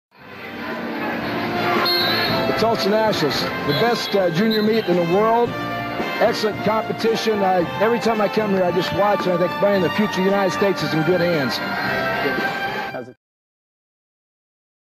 Joe Seay Invterview about WOW Events
World Level Coach, Joe Seay, talks about the impact of WOW events.